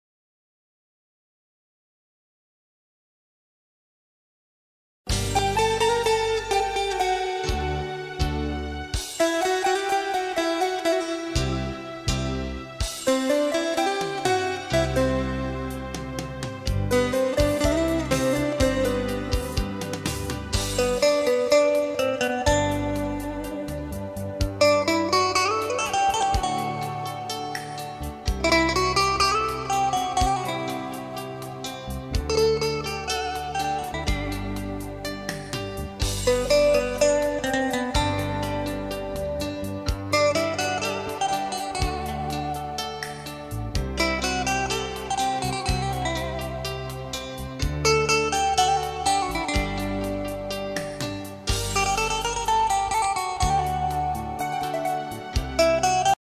Pixel LED Tabela - Game Plus T.Ç.
Pixel LED Tabela - Game Plus Proje Detayları
Zemin : 2,7m çapında siyah alüminyum komposit panel
Animasyon: 25 adet 1m'lik 144 piksel dijital tüp